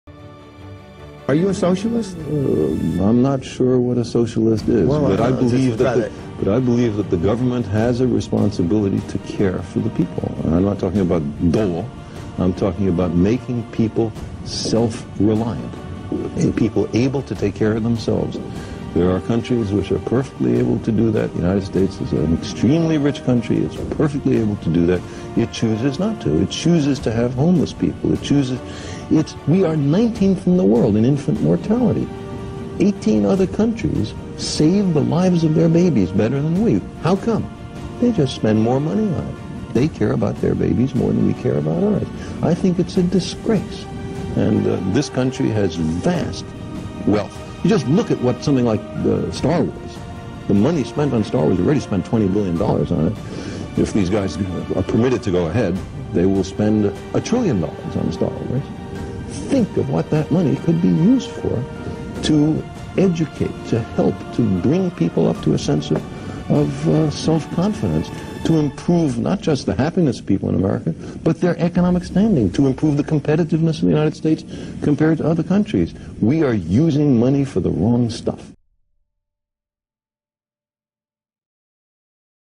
Ted Turner asks Carl Sagan "Are you a socialist?"
Carl Sagan speaking on CNN in 1989 about American wealth and the misuse of money on programs like star wars instead of addressing societal problems like homelessness and infant mortality.